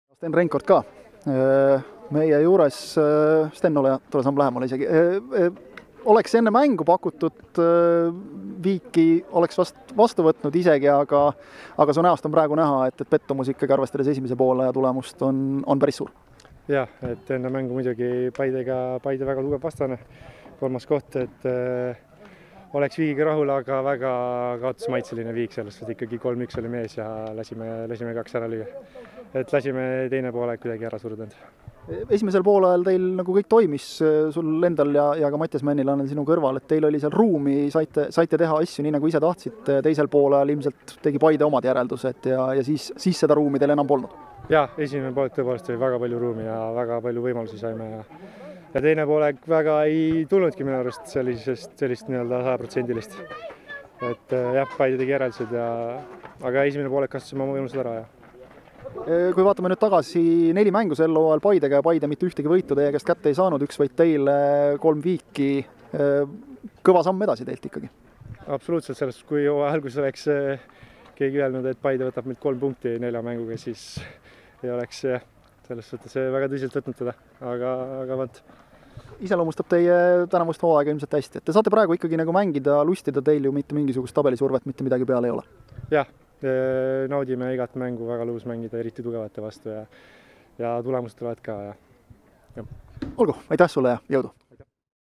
FC Kuressaare - Paide Linnameeskond l 3:3 l Premium liiga 31. voor l 2022 | Reinkorti intervjuu
9. oktoobril kell 14.30 Kuressaare linnastaadionil

FC Kuressaare - Paide Linnameeskond 3:3 (3:1)
10. Mattias Männilaan, 32., 40. Sten Reinkort - 16. Mechini Gomis, 49. Robi Saarma, 59. Kristofer Piht